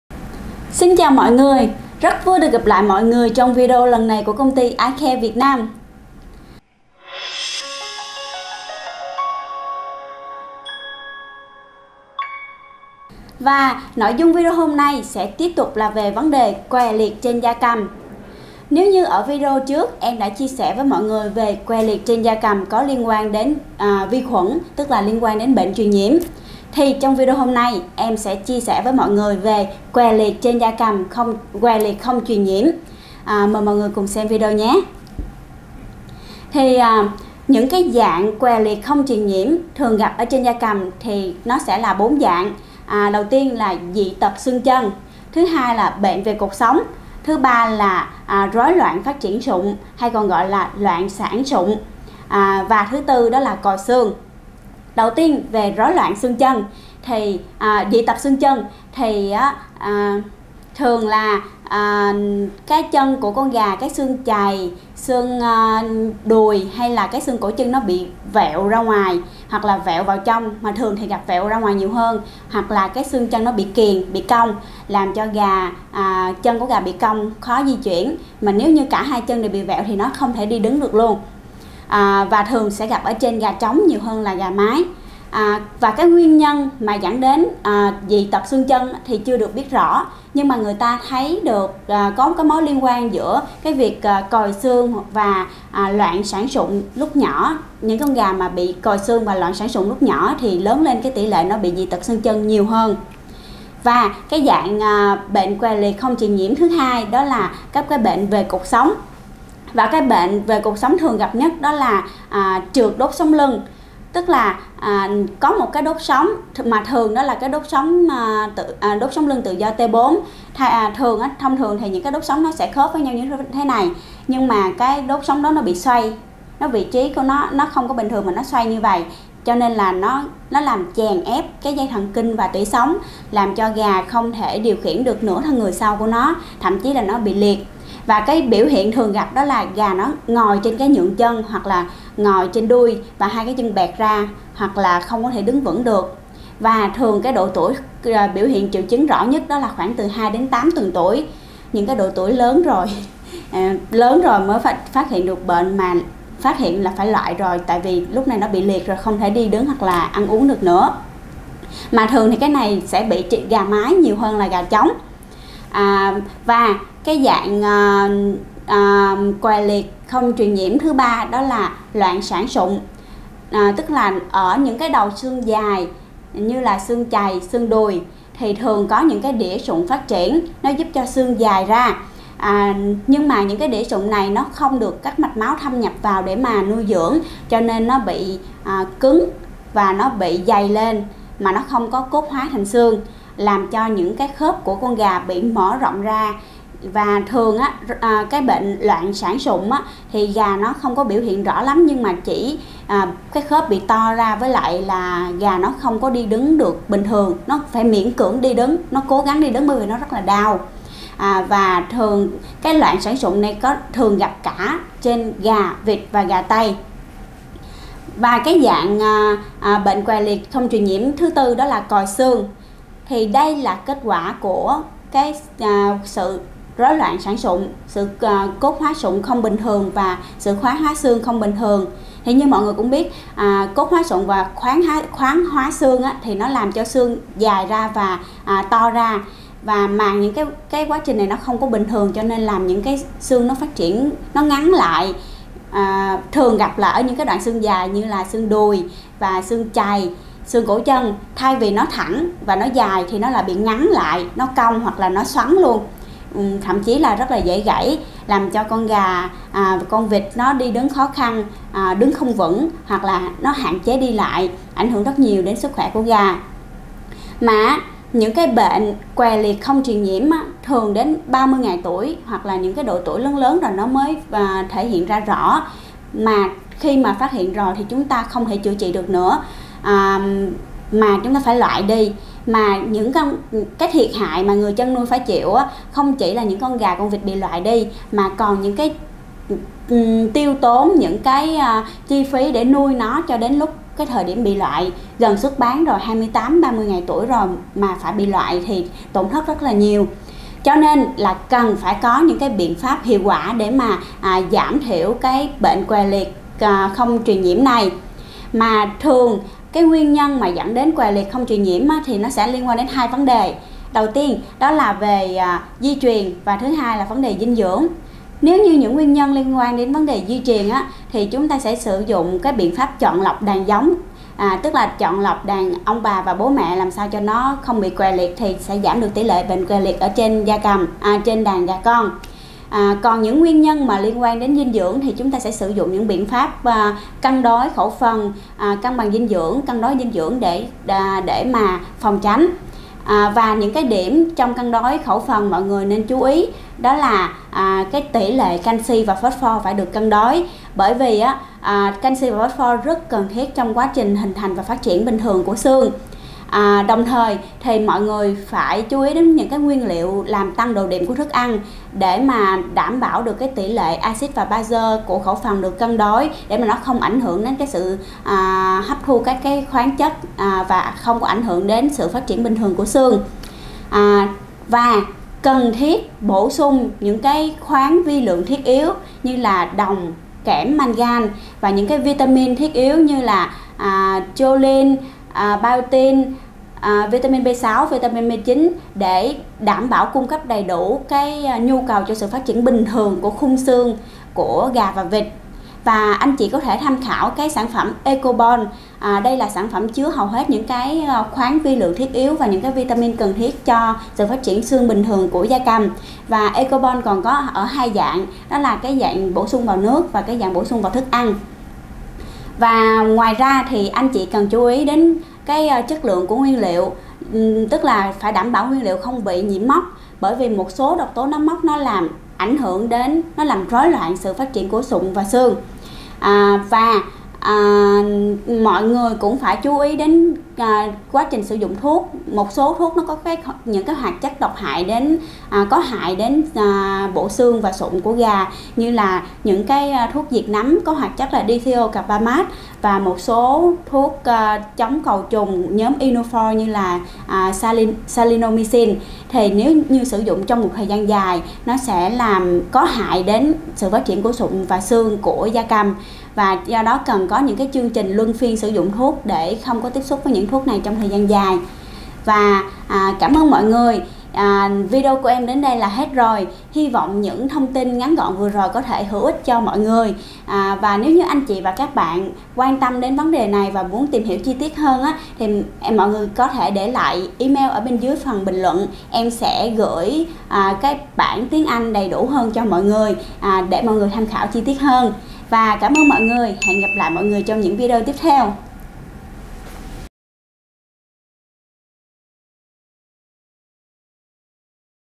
BỆNH QUÈ LIỆT KHÔNG TRUYỀN NHIỄM TRÊN GIA CẦM VÀ CÁCH KHẮC PHỤC
Bệnh què liệt không truyền nhiễm trên gia cầm thường do các nguyên nhân về rối loạn xương. Bệnh gây thiệt hại kinh tế tương đối lớn với người chăn nuôi. Có 3 nhóm bệnh là: Dị tật xương chân, trượt đốt sống lưng, loạn sản sụn và còi xương.
Mời bạn tham khảo chi tiết cách khác phục trong video này